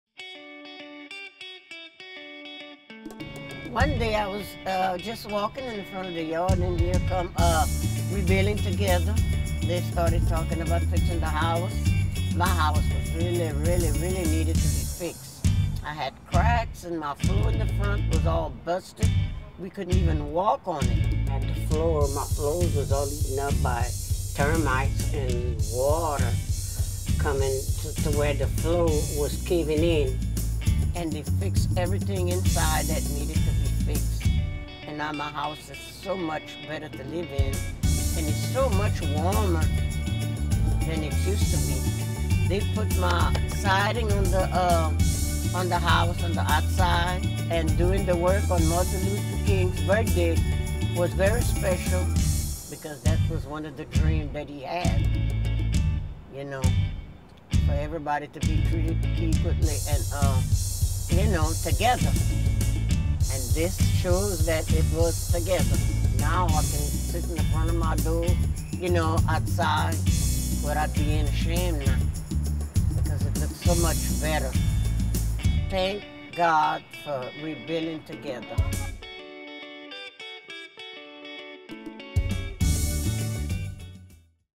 Miss Mary
Miss Mary is a longtime resident of her Lafayette, Louisiana, home. In 2015, Rebuilding Together's AmeriCorps members helped make her home safer and healthier, in honor of Dr. Martin Luther King Jr. Day of Service.

This year, we caught up with Miss Mary to see how the repairs have helped transform her life.